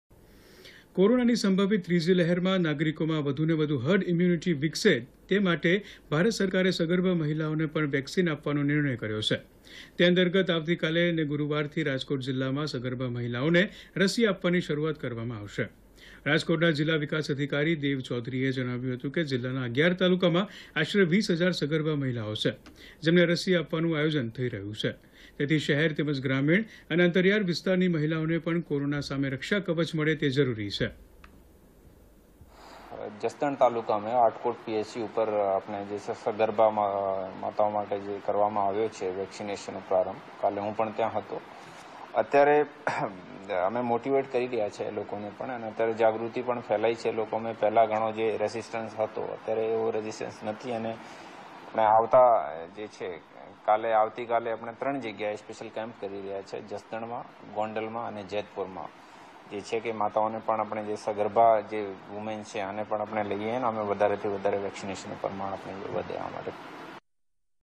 રાજકોટ જિલ્લામાં આગામી ગુરૂવારથી સગર્ભા મહિલાઓને અપાશે વેક્સિન
કોરોનાની સંભવિત ત્રીજી લહેરમાં નાગરિકોમાં વધુને વધુ Herd Immunity વિકસી રહે તે માટે ભારત સરકારે સગર્ભા મહિલાઓને પણ વેક્સિન આપવાનો નિર્ણય કર્યો છે. તે અંતર્ગત આવતીકાલને ગુરૂવારથી રાજકોટ જિલ્લામાં સર્ગભા મહિલાઓને, રસી આપવાની શરૂઆત કરવામાં આવશે. રાજકોટના જિલ્લા વિકાસ અધિકારી દેવ ચૌધરીએ જણાવ્યું હતું, કે જિલ્લાના 11 તાલુકામાં આશરે 20 હજાર સગર્ભા મહિલાઓ છે, જેમને રસી આપવાનું આયોજન થઈ રહ્યું છે. તેથી શહેર તેમજ ગ્રામિણ અને અંતરિયાળ વિસ્તારની મહિલાઓને પણ કોરોના સામે રક્ષાકવચ મળે તે જરૂરી છે.